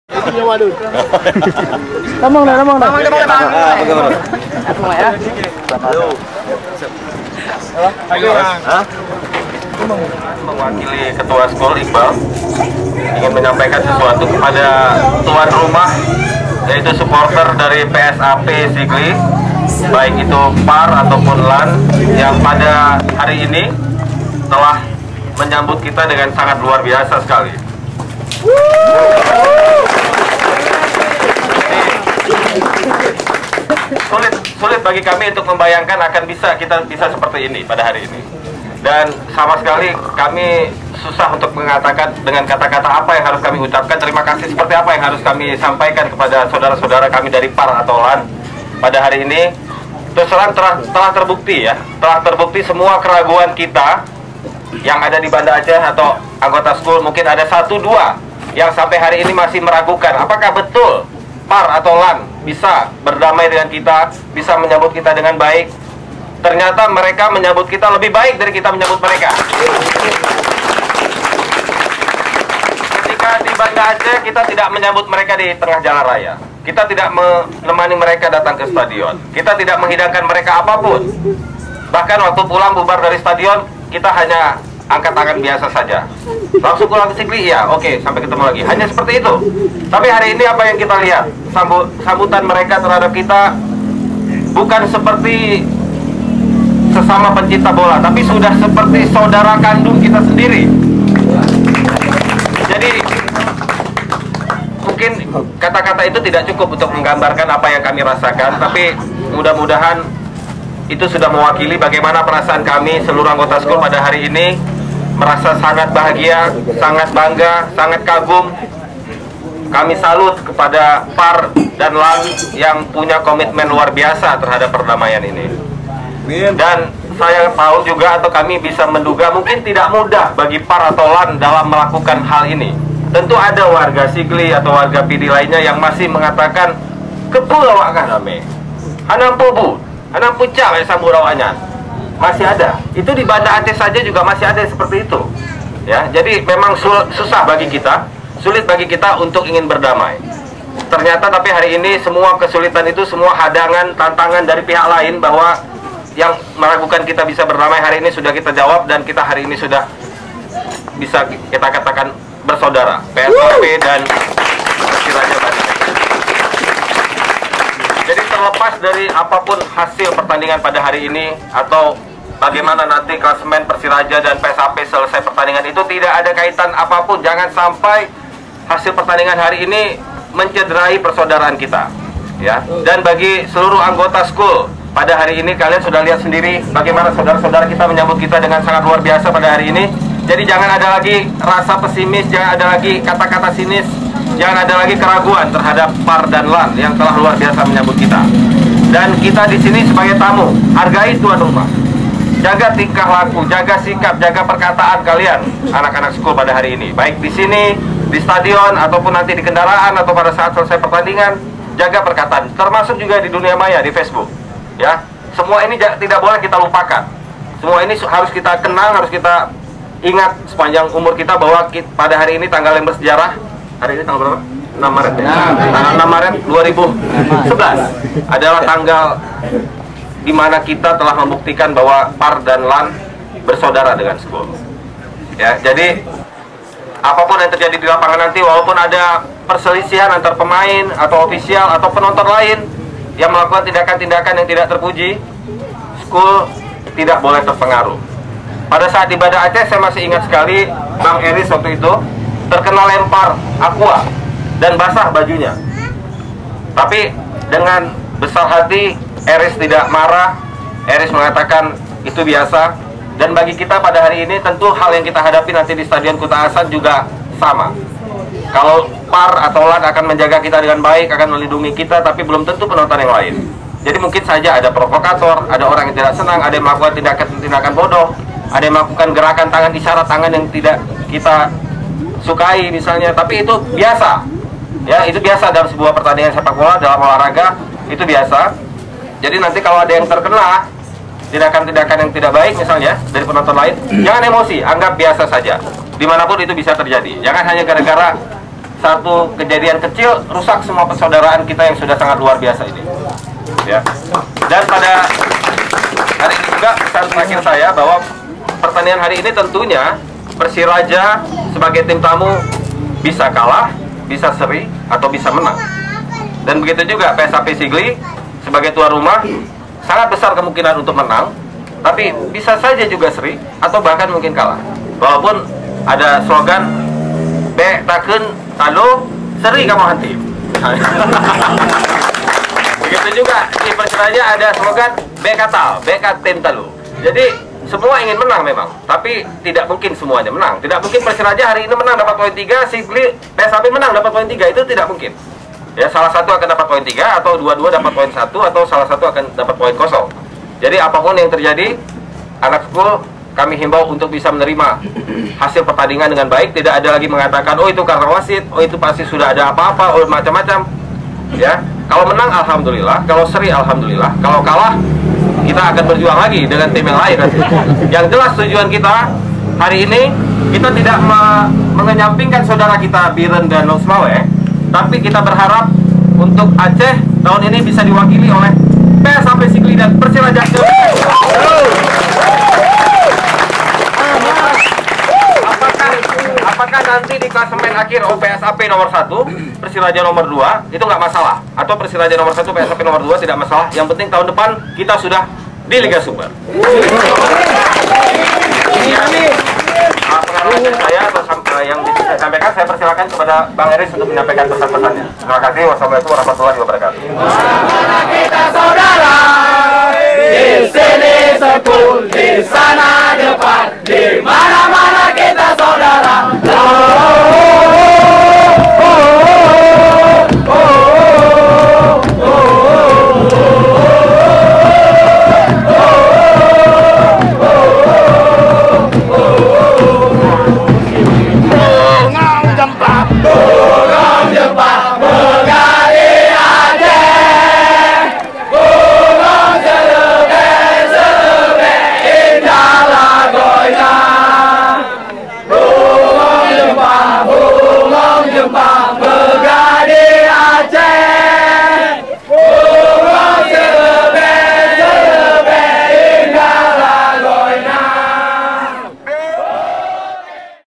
Pidato Perdamaian Suporter Persiraja dan PSAP di Sigli .m4v
Setelah sempat bentrok dan rusuh hebat di Stadion Harapan Bangsa pada musim kompertisi Divisi Utama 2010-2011, Suporter Persiraja (SKULL) dan Suporter PSAP (PARR) menyatakan ikrar damai dan menjalin persaudaraan.
Dalam Video ini terlihat Penasehat SKULL Teuku Irwan Djohan memberi kata sanbutan di Markas PARR di Sigli 6 Maret 2011